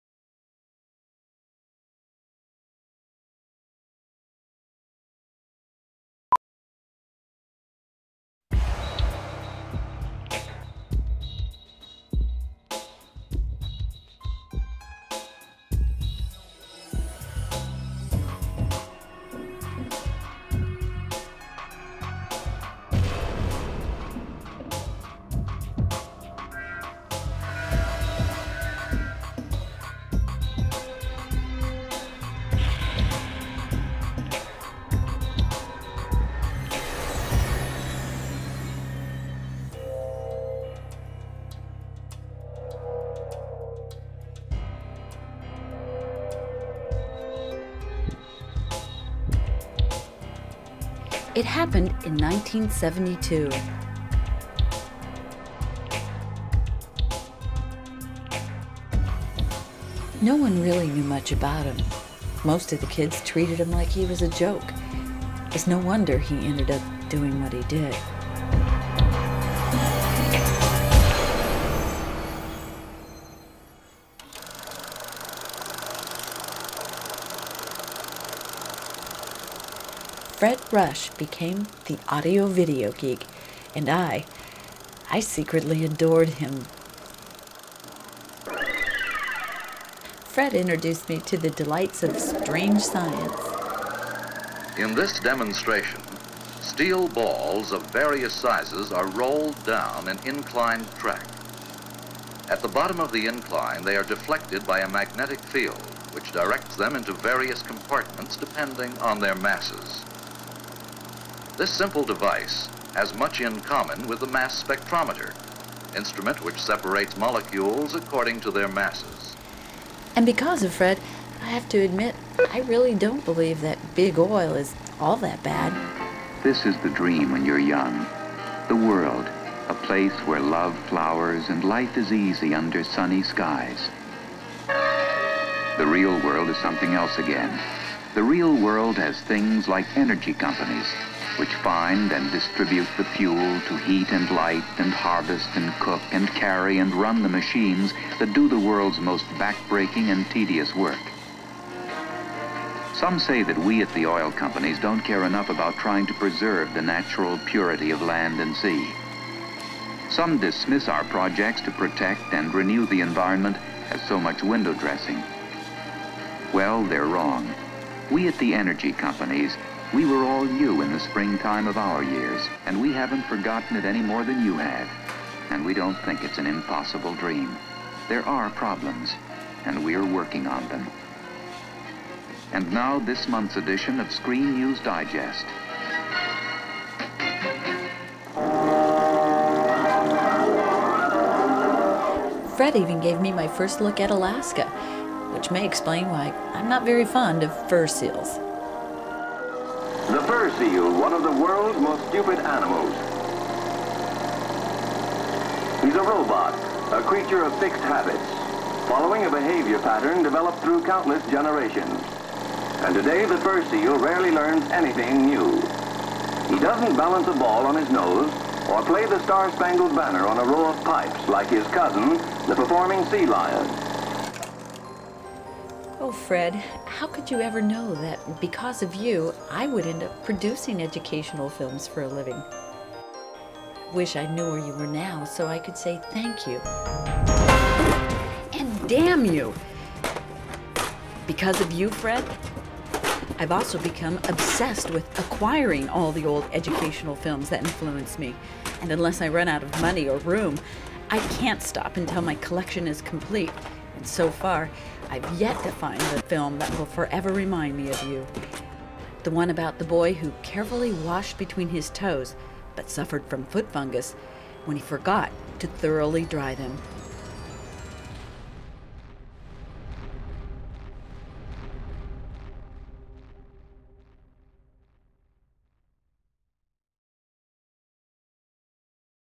Extreme Obsession
This is dedicated to those of us of a certain age who spent our Wonder Years  in dark classrooms watching those hokey educational films that teachers would spool up to scare or indoctrinate us while they went to the lounge to cop a smoke.

This film is about what that led to in my life decades later. This is a true story. Honest.

AND...if you like to collect 16mm educational films, I know where you can get some.  My less-than-understanding family has guilted me into letting go of a few of my treasures on ebay. I don't understand the big deal. No one was using the basement anyway.